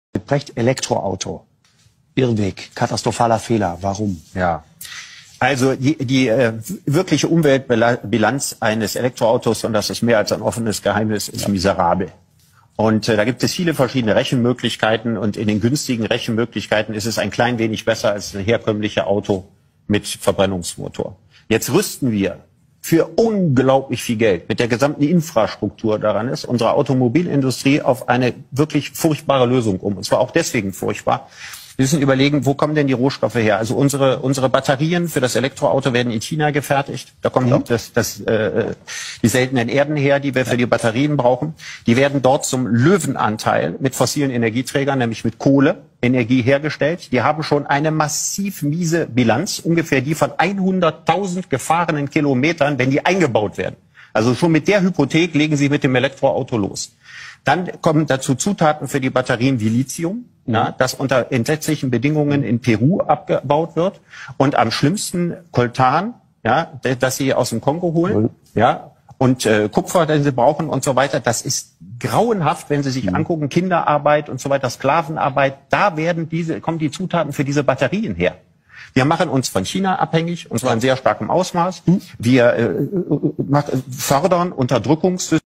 Die Wahrheit über E-Mobilität von Richard David Precht
Richard David Precht ist aus vielerlei Gründen angesehen. Mit seinen offenen Worten zur E-Mobilität öffnet er vielen die Augen. Denk mal selbst darüber nach!